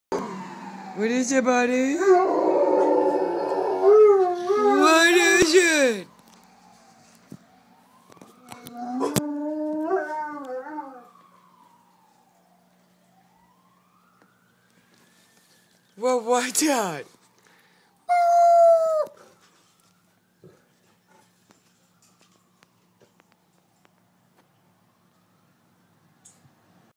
Leonidas hates sirens
Leo my 130 lb Mastador hates sirens.